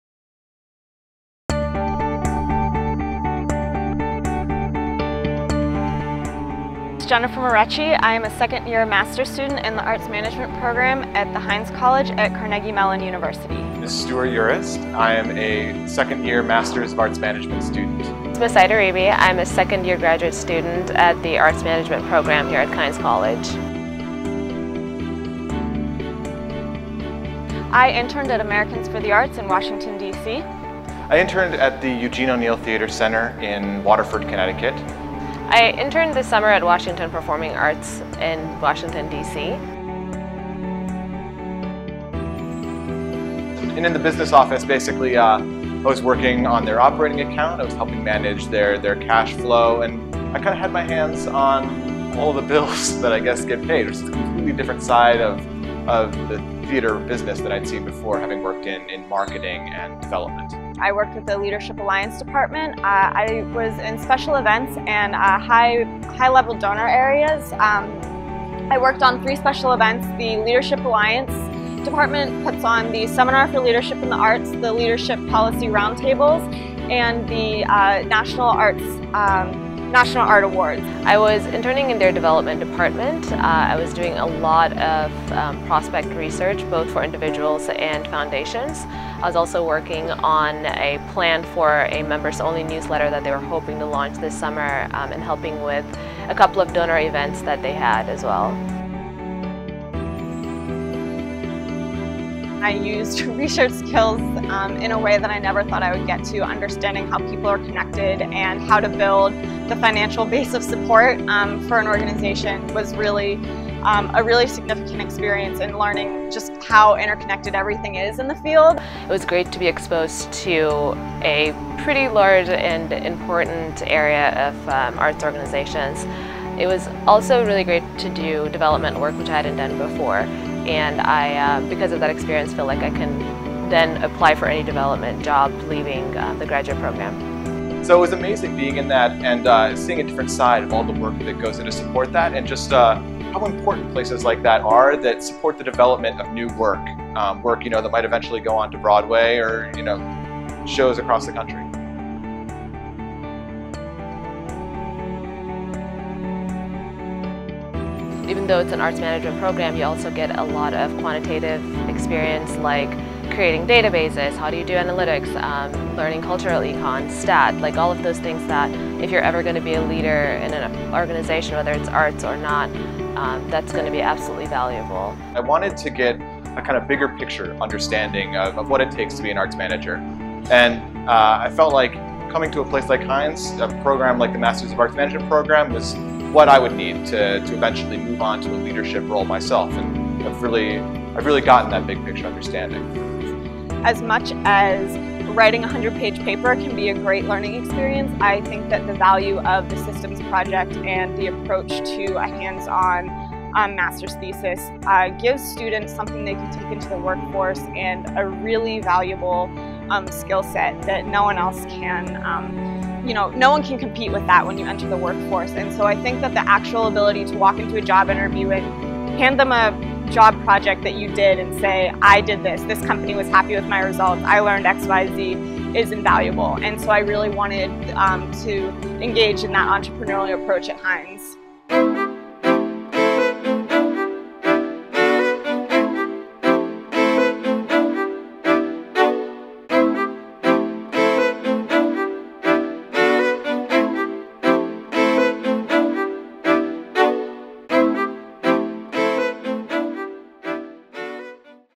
Masters of Arts Management Intern Experience
Three Master of Arts Management (MAM) students from Carnegie Mellon University's Heinz College of Information Systems and Public Policy go over their internships and why the MAM program offers a unique approach to their graduate education.